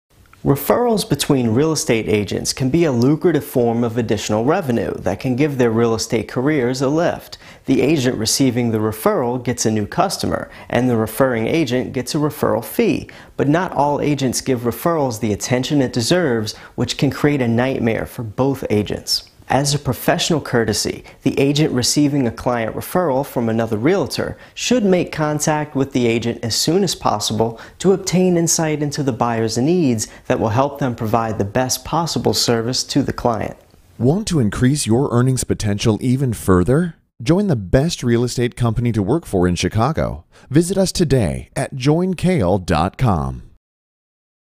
Best Way to Avoid Agent Referral Issues
- Referrals between real estate agents can be a lucrative form of additional revenue for both the sender and the receiver that can give their real estate career a lift.